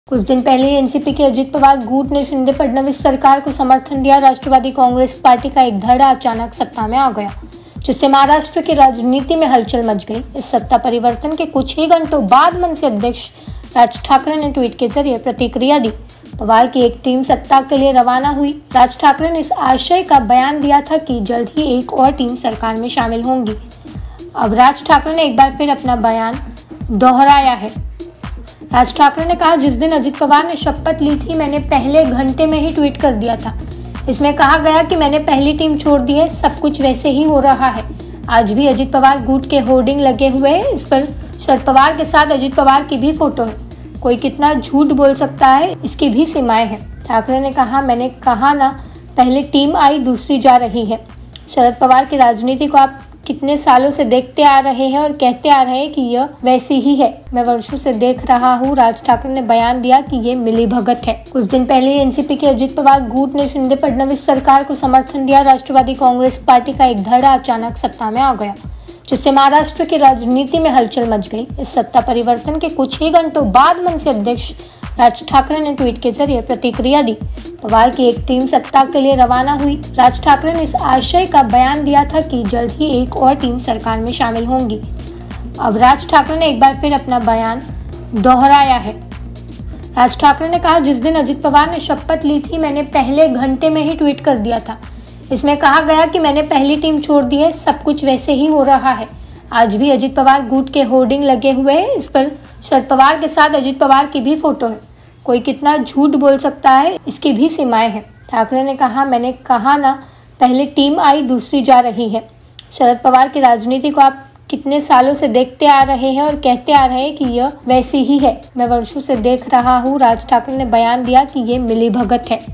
Deputy CM Ajit Pawar पर मनसे अध्यक्ष राज ठाकरे ने फिर से निशाना साधा
कुछ दिन पहले ही एनसीपी के अजित पवार गुट ने शिंदे-फडणवीस सरकार को समर्थन दिया है. राष्ट्रवादी कांग्रेस पार्टी (एनसीपी) का एक धड़ा अचानक सत्ता में आ गया, जिससे महाराष्ट्र की राजनीति में हलचल मच गई. इस सत्ता परिवर्तन के कुछ ही घंटों बाद मनसे अध्यक्ष राज ठाकरे ने ट्वीट के जरिए प्रतिक्रिया दी. पवार की एक टीम सत्ता के लिए रवाना हो गई है. राज ठाकरे ने इस आशय का बयान दिया था कि जल्द ही एक और टीम सरकार में शामिल होगी. अब राज ठाकरे ने एक बार फिर अपना बयान दोहराया.राज ठाकरे ने कहा, ''जिस दिन अजित पवार ने शपथ ली थी, मैंने पहले घंटे में ही ट्वीट कर दिया था. इसमें कहा गया कि मैंने पहली टीम छोड़ दी है. सब कुछ वैसे ही हो रहा है. आज भी अजित पवार गुट के होर्डिंग लगे हुए हैं. इस पर शरद पवार के साथ अजित पवार की भी फोटो है. कोई कितना झूठ बोल सकता है इसकी भी सीमाएं हैं.” ठाकरे ने कहा, मैंने कहा न, पहली टीम आई, दूसरी जा रही है. शरद पवार की राजनीति को आप कितने सालों से देखते आ रहे हैं और कहते आ रहे हैं कि यह वैसी ही है? मैं वर्षों से देख रहा हूं. राज ठाकरे ने बयान दिया है कि ये मिलीभगत है.